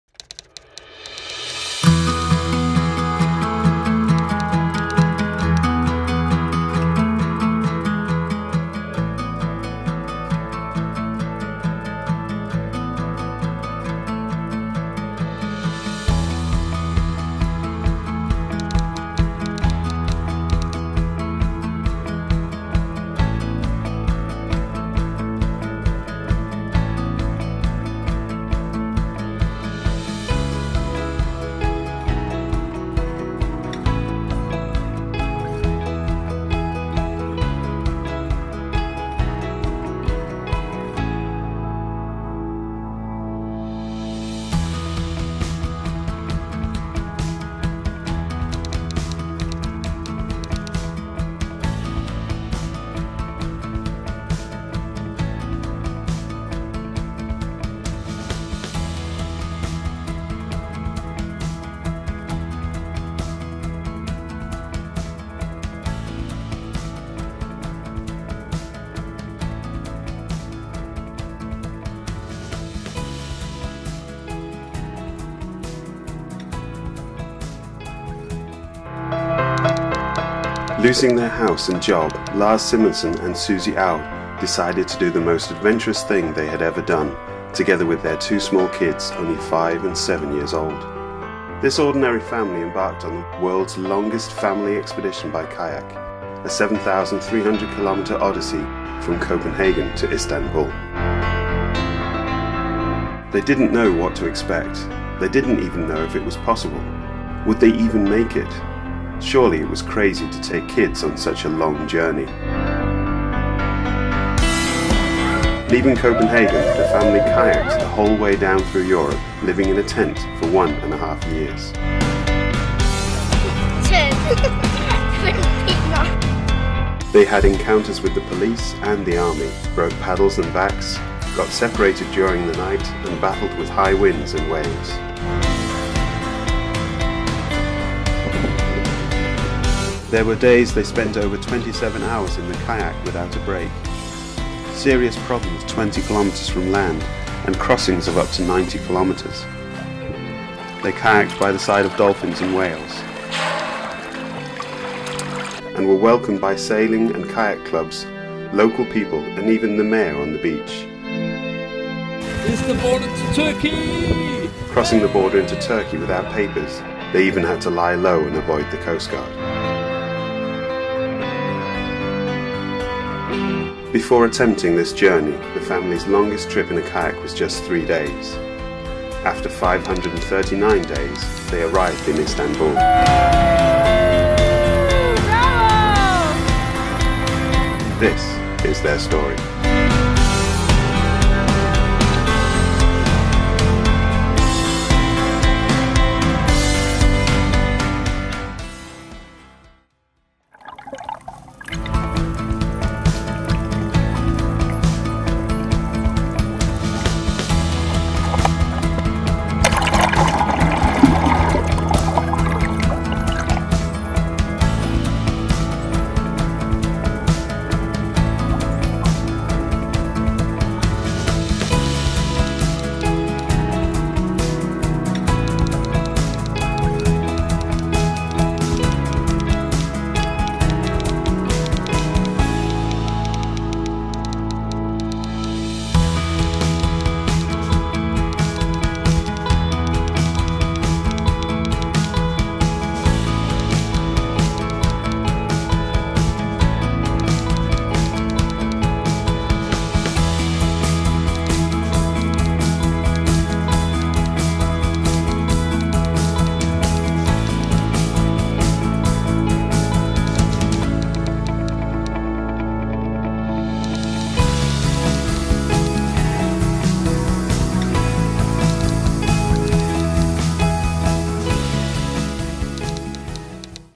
The Hobie Mirage Tandem Island Promo
The ultimate boat for explorers.  The Tandem Island is ideal for exploring rivers, estuaries, harbours, lakes and sea.  Stable, fast, fun. and really easy to rig up and launch.  Come and have a go in Poole Harbour.

We have taken the Boat Show online in 2019.  For a limited time only starting 24th April 2019 you can explore the boats and learn more about all their features by touring our Virtual Boat Show.

Thanks to Lars Simonsen for the footage follow his adventures at @lifeisgoodfollowus